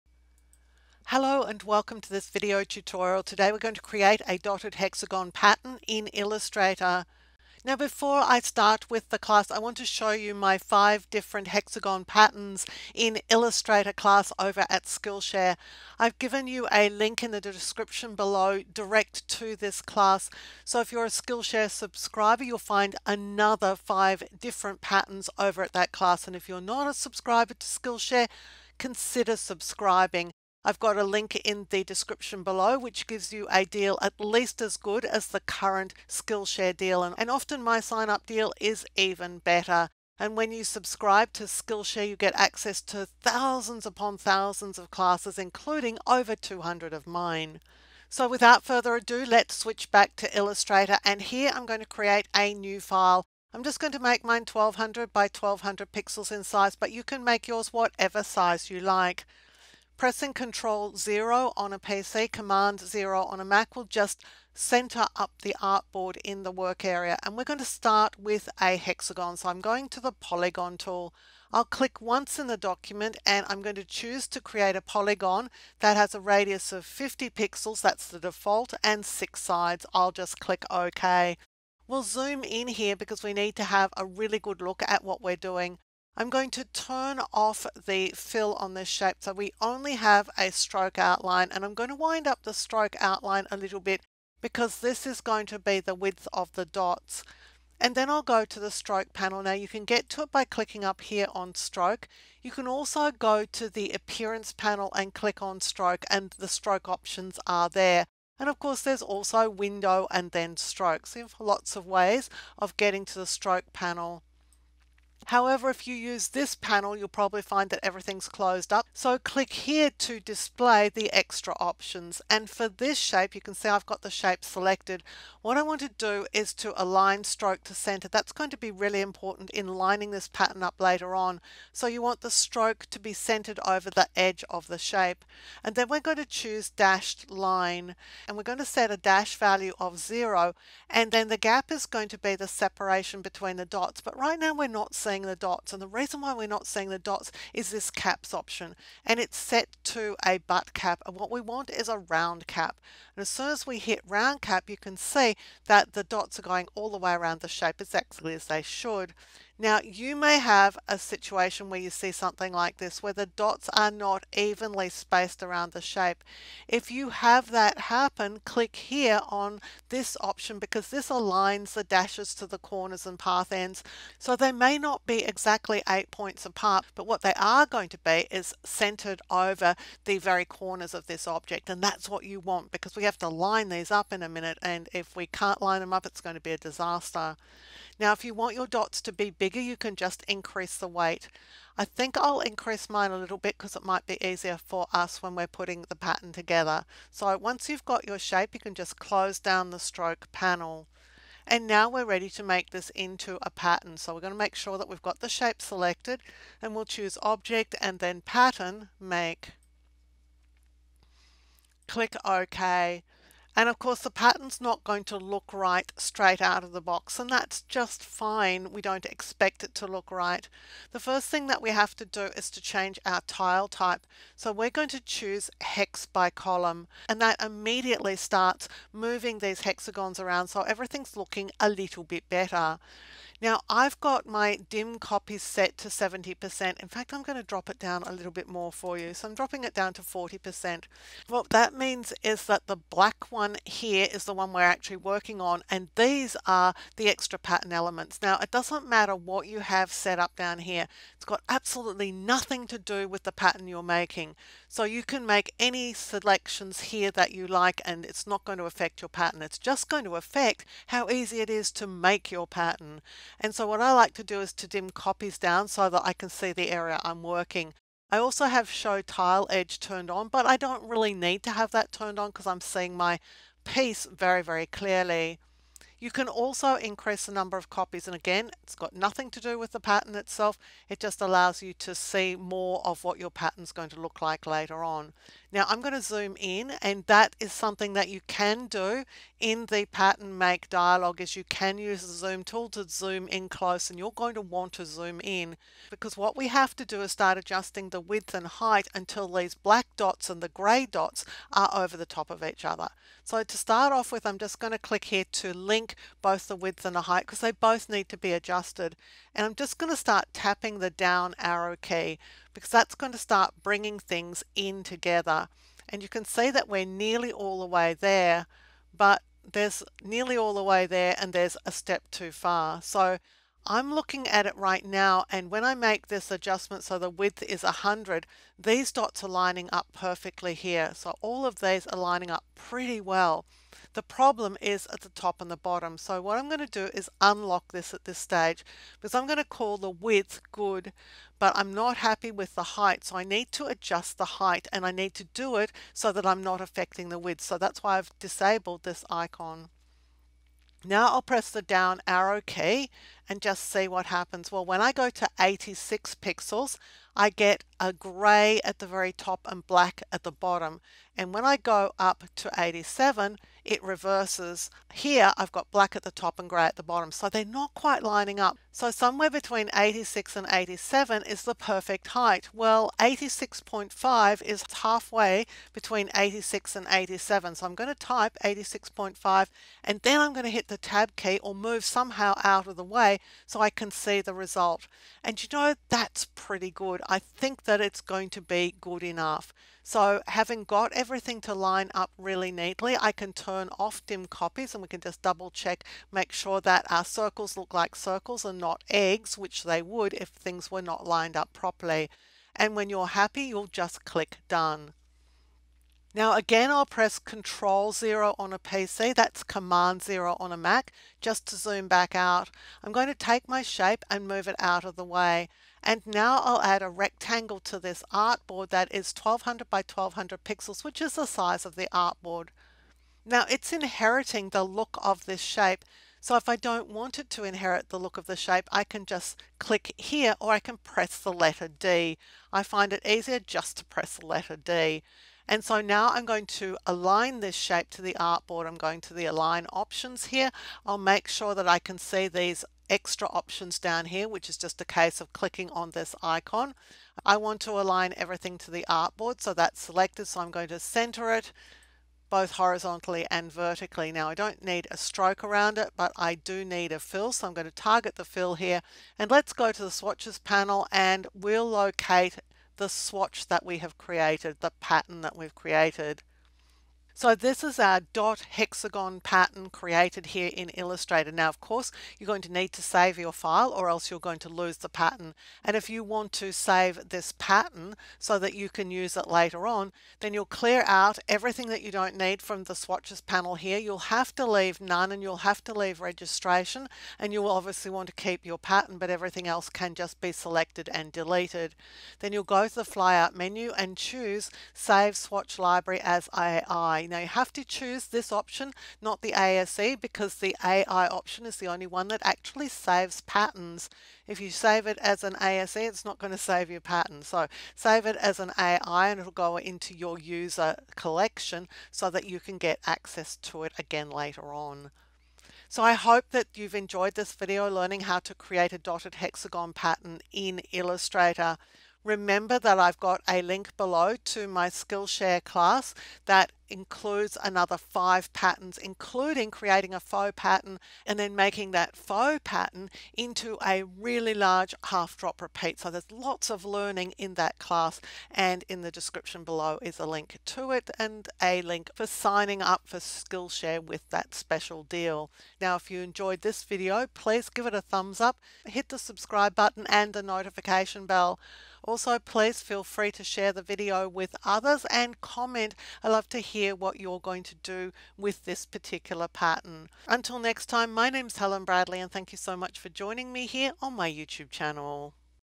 Illustrator - Dotted Hexagon Pattern - Create seamless repeating patterns with ease
Create a dotted hexagon pattern.
Learn to create a hexagon shape with a dotted stroke and then turn that into a seamless repeating pattern. You will learn valuable Illustrator tools and techniques in this video that you can use everyday.